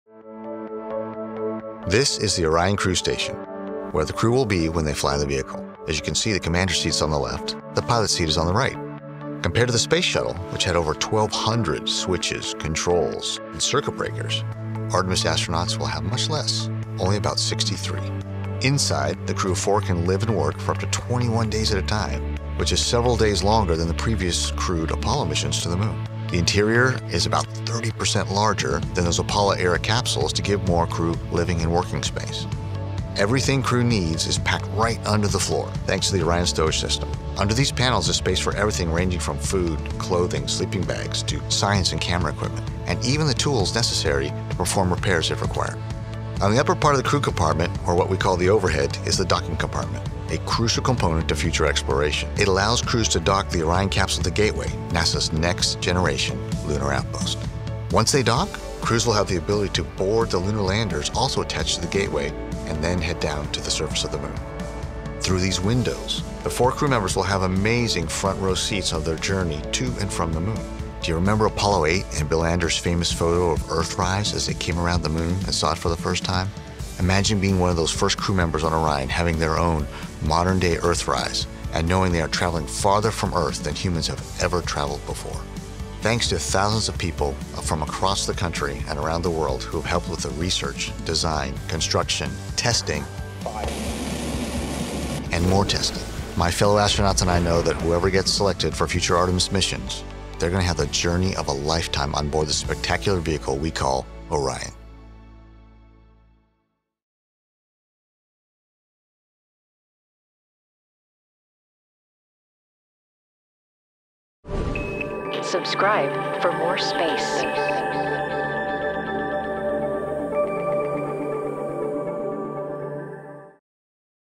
Explore the Orion Crew Capsule with Astronaut Randy Bresnik
The focus of the flight of Artemis I is to test Orion’s systems ahead of crewed missions.  Starting with Artemis II, human crew members will be on board. You may be thinking, “what is it like inside?”  I’m glad you asked… follow along as NASA astronaut Randy Bresnik guides us through the interior of the Orion spacecraft and a close up look at elements that will support a crew on humankind’s next trip to the Moon.